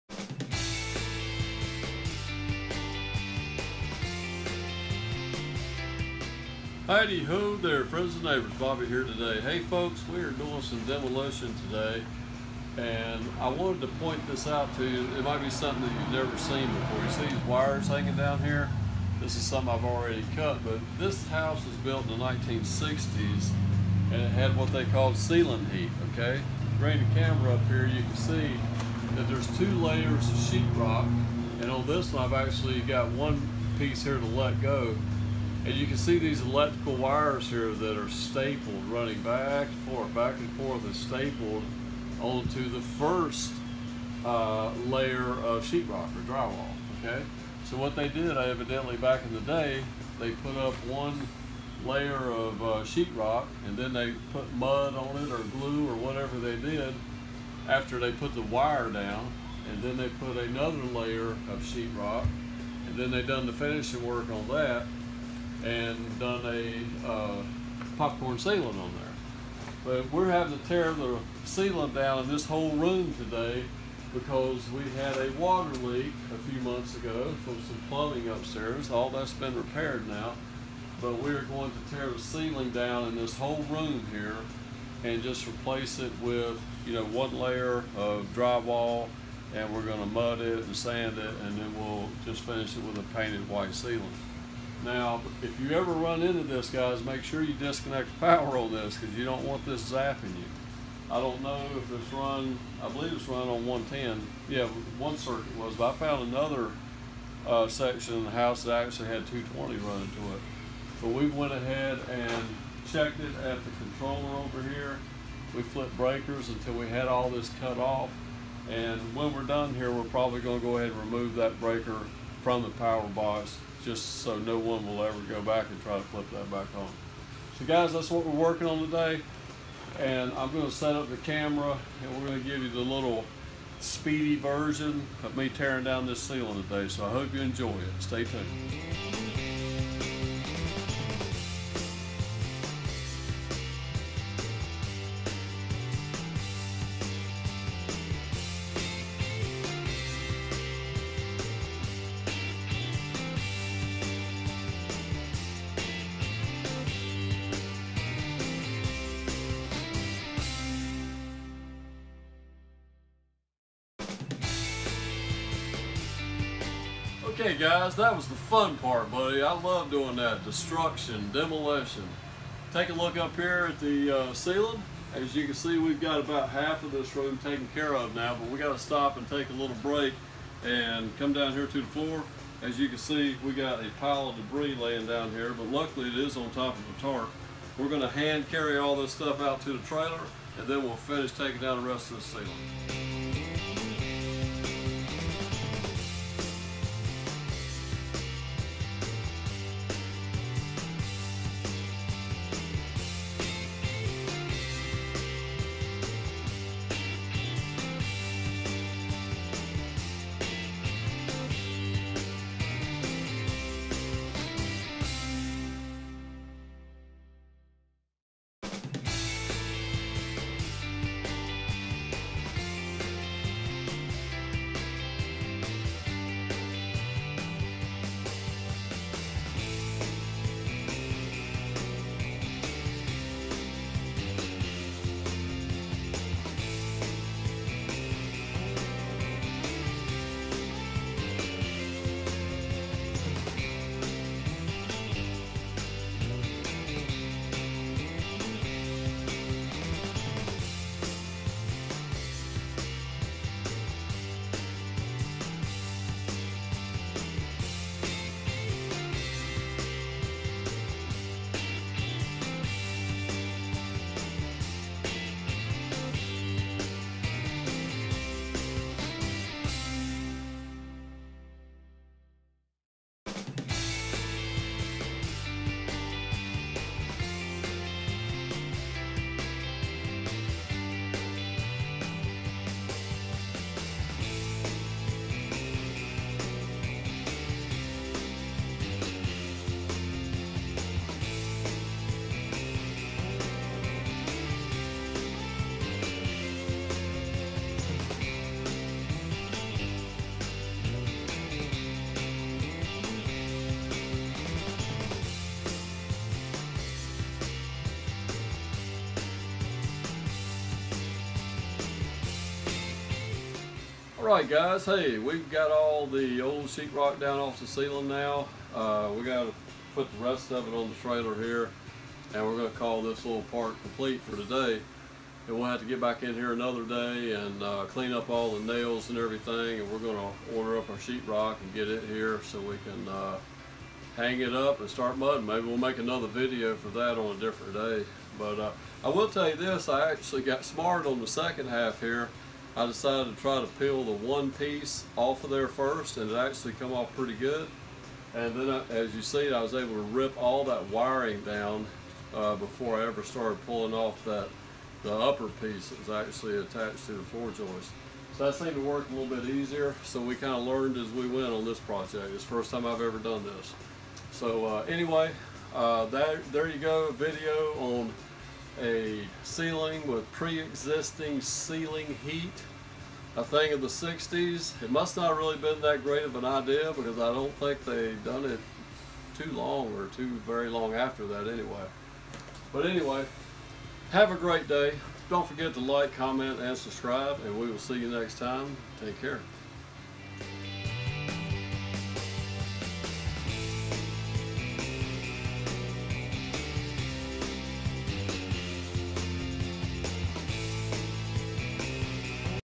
Ceiling Heat Removal / Demo / Clean-Up - How to - DIY
TOOLS You Will NEED to DEMO a Heated Ceiling
Mattock 2.5 lb
Hard Hat
Dust Mask
Safety Goggles

Check out our removal of an old 1960's style heated ceiling. If you choose to remove this style of ceiling please make sure the power to the ceiling has been disabled.

for as little as $1 per month